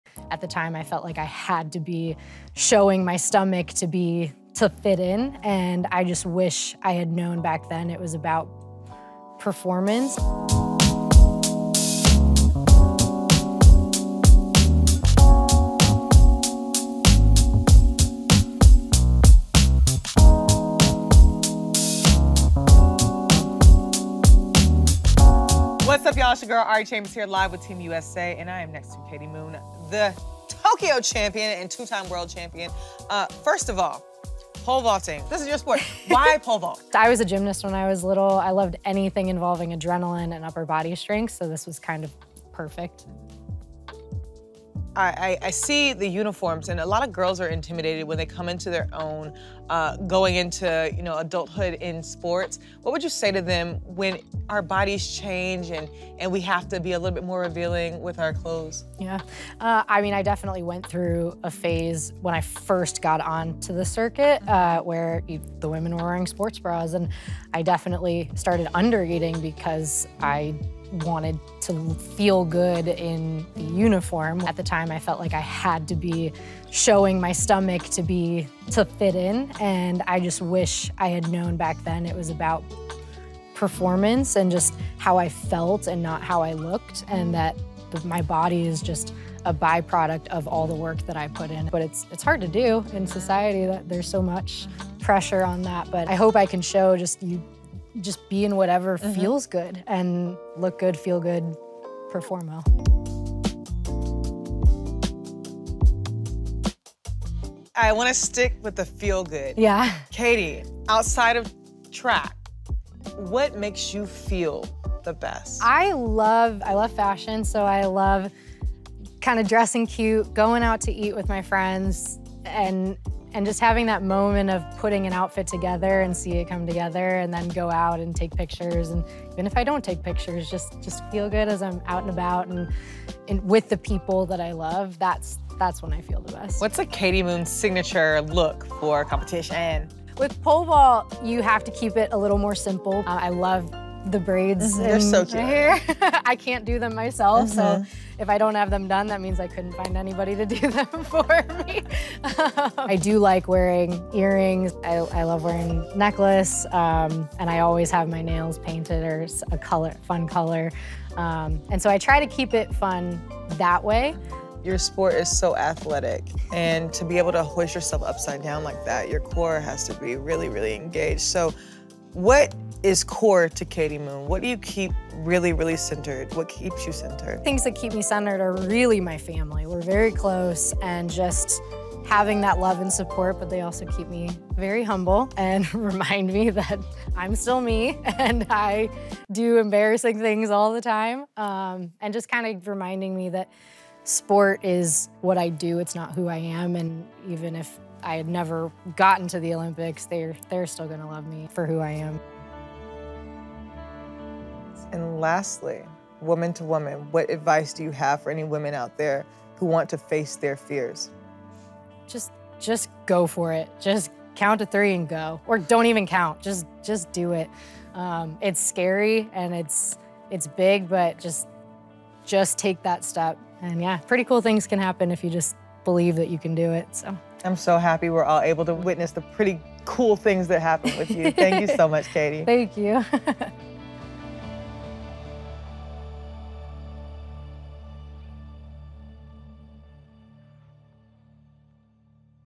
Woman to Woman: Olympic gold medalist Katie Moon sits down for an open and honest conversation
Keep soaring, Katie Moon 👏 The reigning Olympic gold medalist in women's pole vault opens up about body image in track and field, her advice to women and more on the road to Paris 2024.